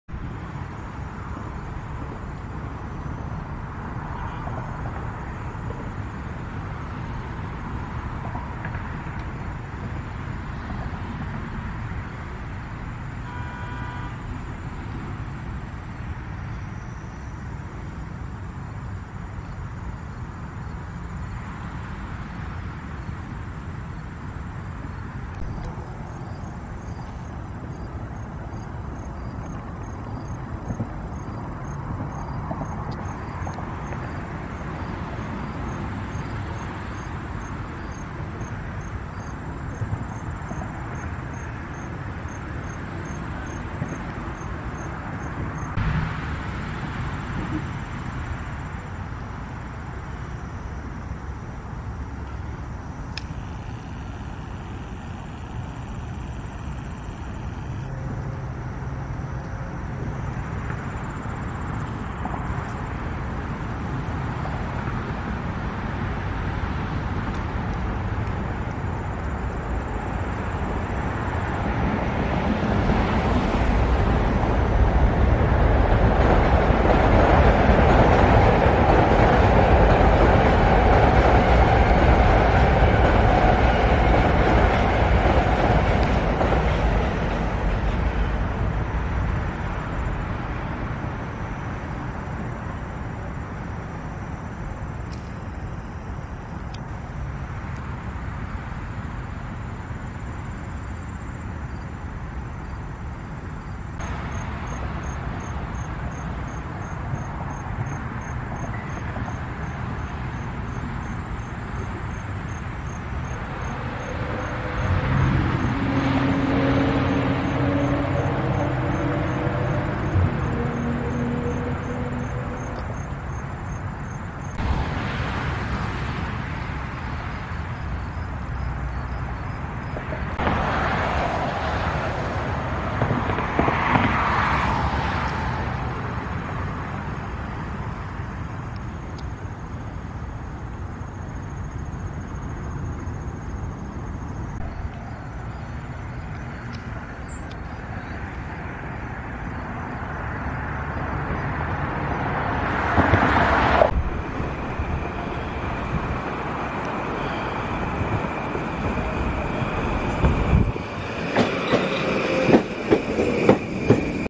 Amazingly Colorful Sunset in Wantagh with Leftover Clouds From Weakening Thunderstorm
Nature has been putting on one heck of a show this weekend, to cap it off, sunset behind an anvil cloud from a weakening thunderstorm in Wantagh. There was just some very light rain, but the colors were so breathtaking and vivid. This was at Twin Lakes Preserve in Wantagh and the Wantagh LIRR station.